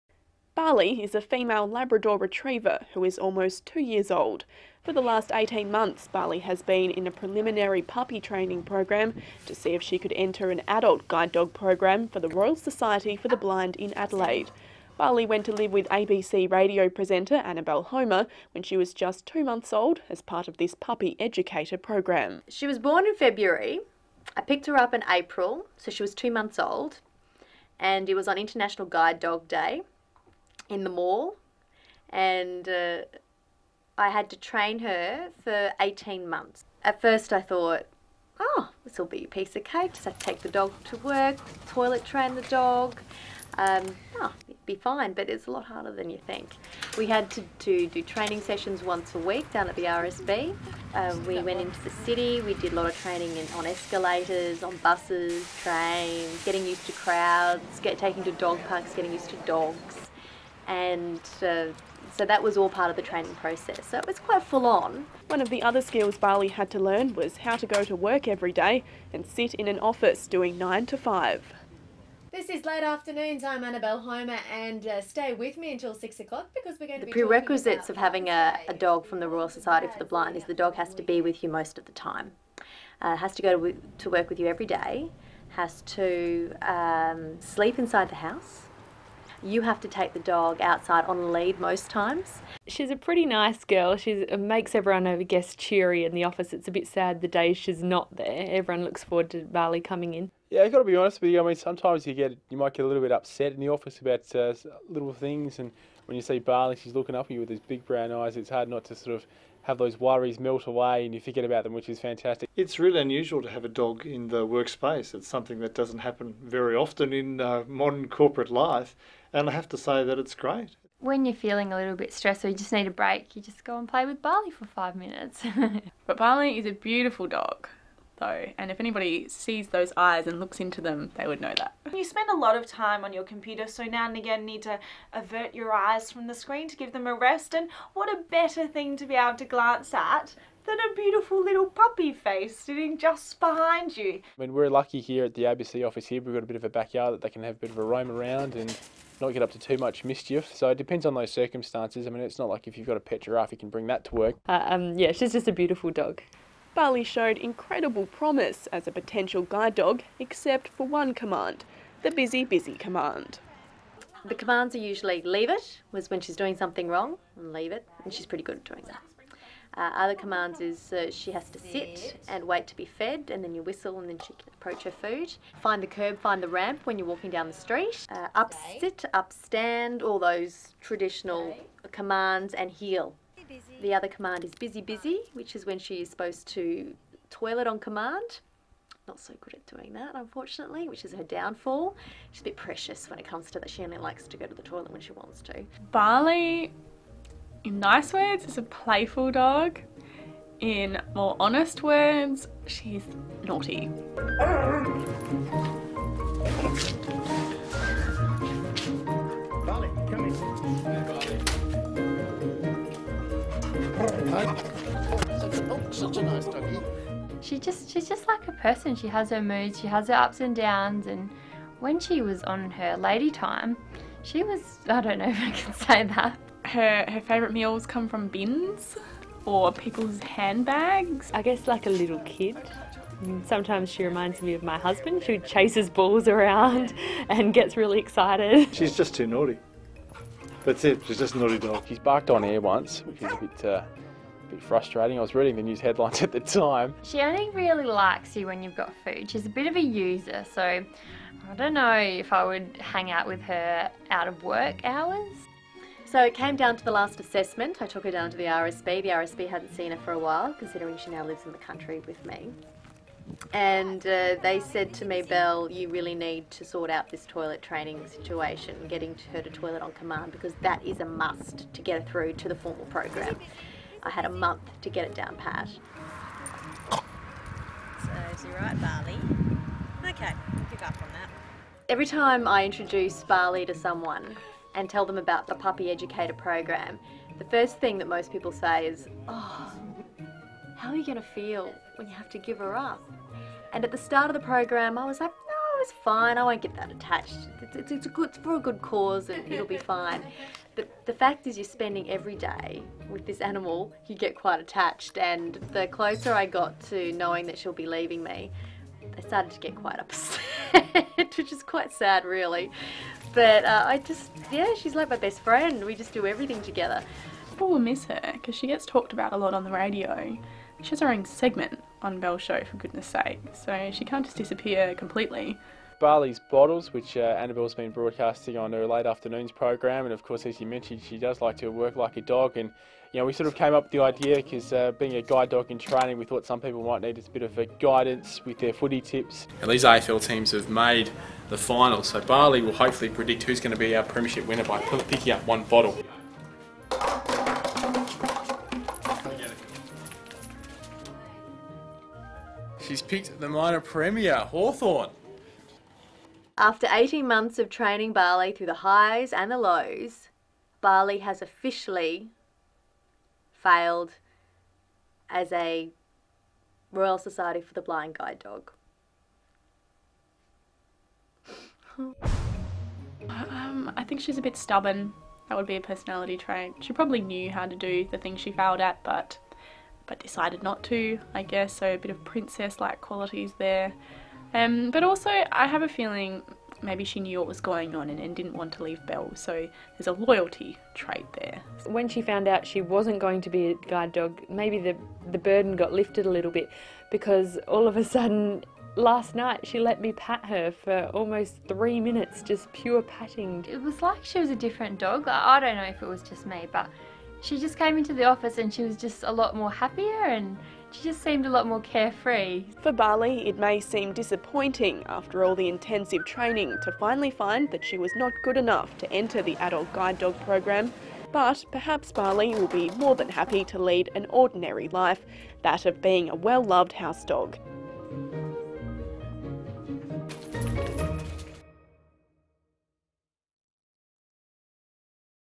Barley, A Very Naughty Working Dog
Have you ever wondered what happens when an RSB Guide Dog pup doesn't pass Puppy Education? Meet Barley!